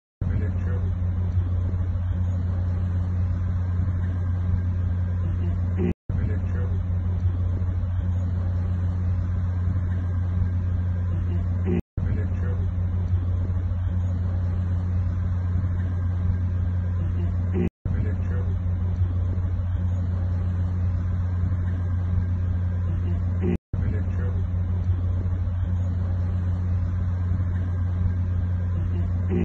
Kim Jong Un की कार ट्रेन में सवार | Russia | North Korea | Viral Video | Trending | N18S
नॉर्थ कोरिया के सुप्रीमो किम की कार उनकी ट्रेन में सवार होती हुई

North Korea's supremo Kim's car boarding his train